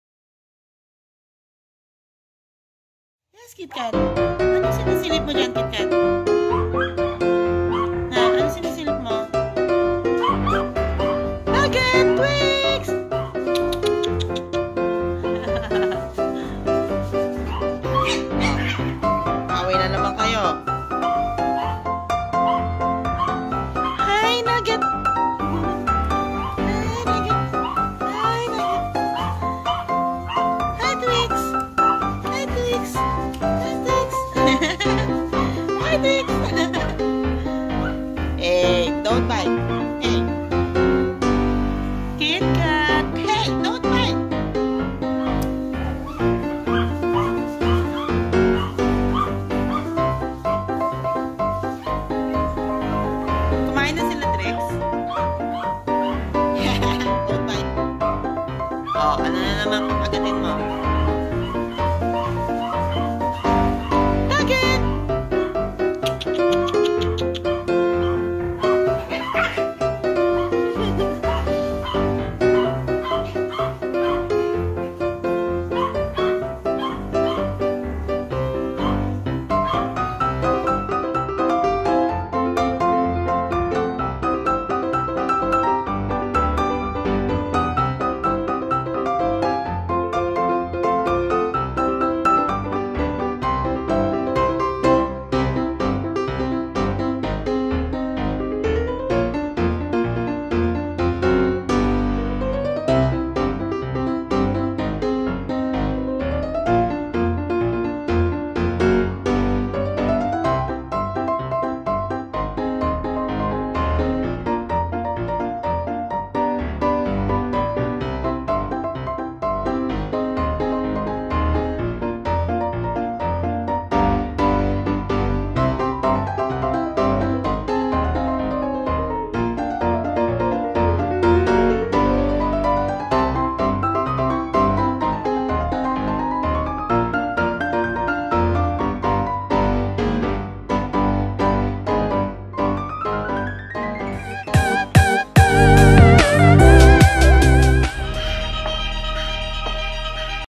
PLAYTIME WITH THE FURBABIES | MEET MY SHIH TZU PUPS - KITKAT, NUGGET AND TWIX
THIS IS WHAT WEEKENDS ARE MADE OF. I PLAY WITH THE FURBEBES AS SOON AS I GO DOWNSTAIRS.

SAD THAT THERE ARE ONLY 3 LEFT FROM 6, BUT IM SURE THEY ARE WITH FORDY NOW.